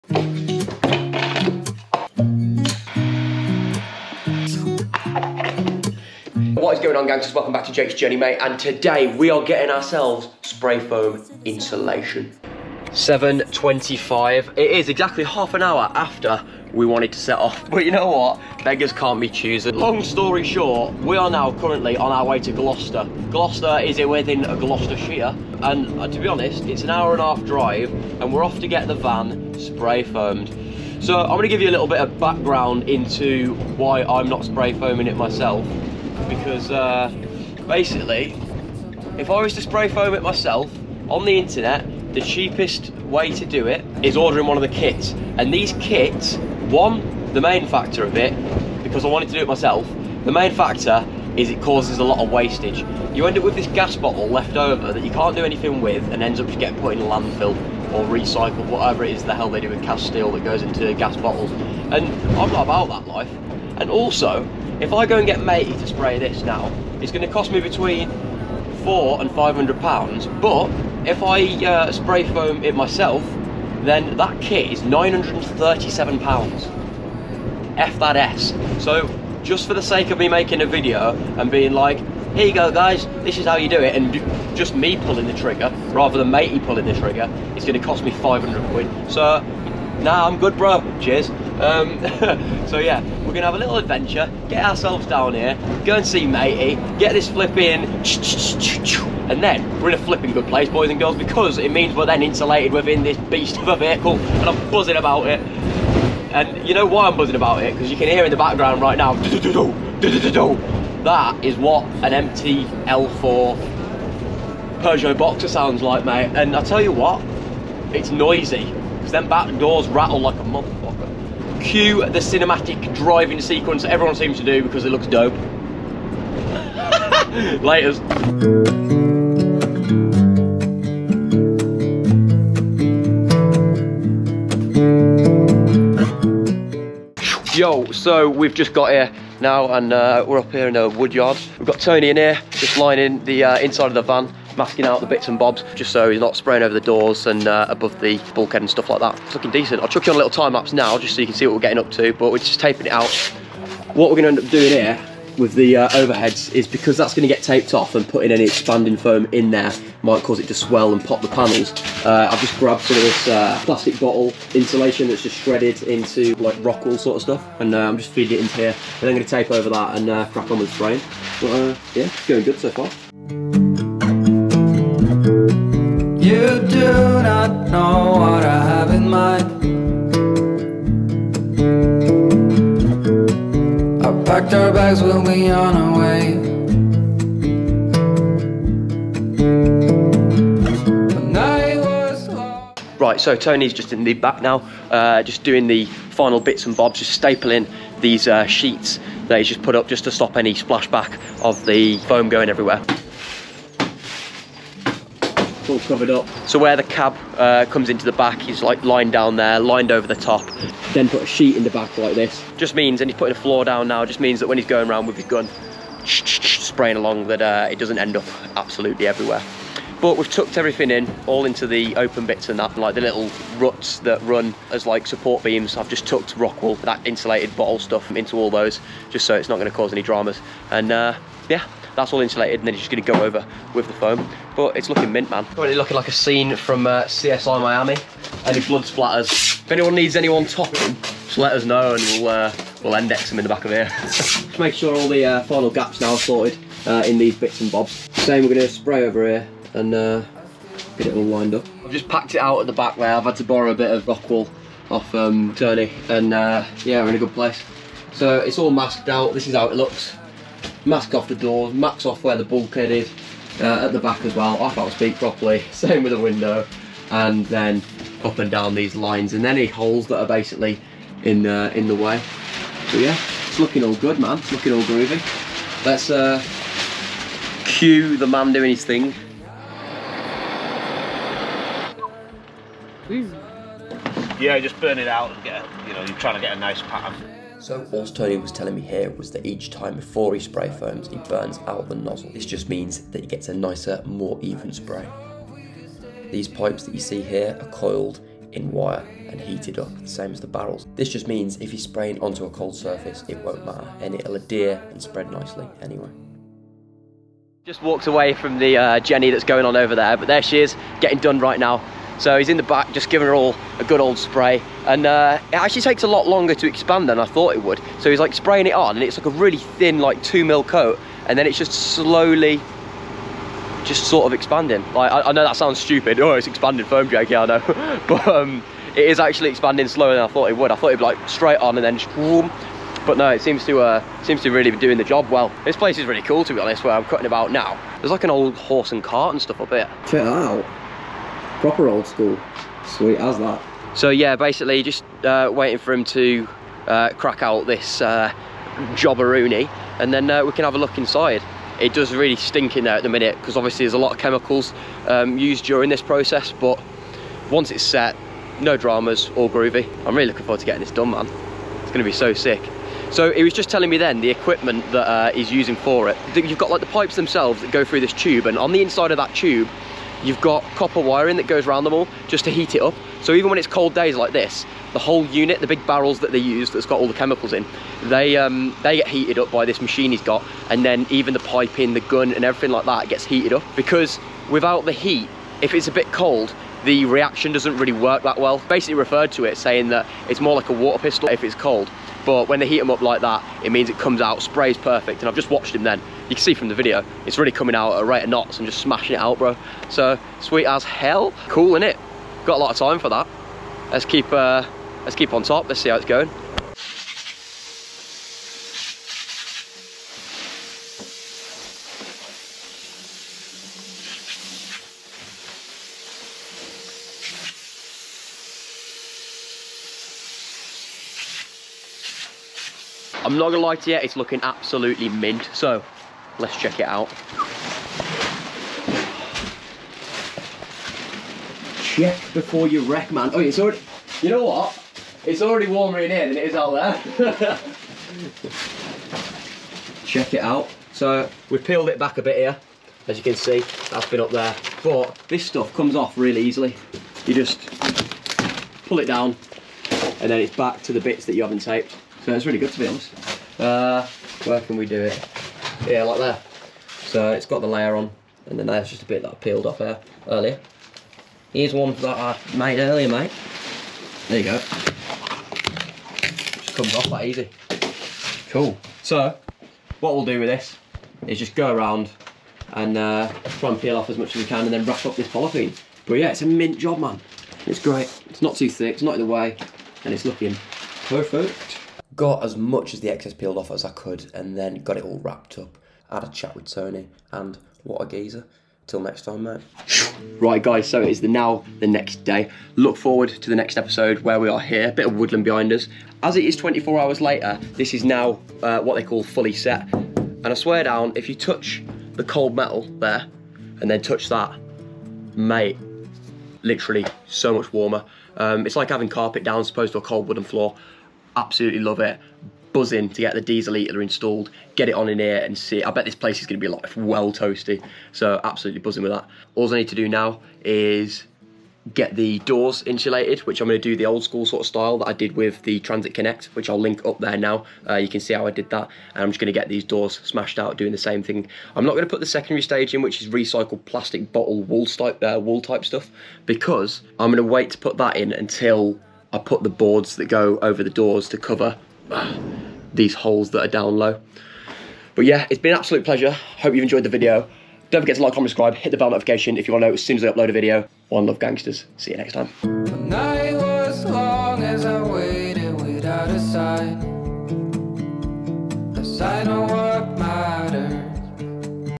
CHEAPEST WAY TO INSULATE A VAN! Extreme Cold weather Insulation!
Wether it's keeping your van cool in the desert or the heat in while skiing in the Alps... This is the insulation for you. With no doubt the best way to insulate a van! closed cell spray foam insulation! The Ultimate van insulation!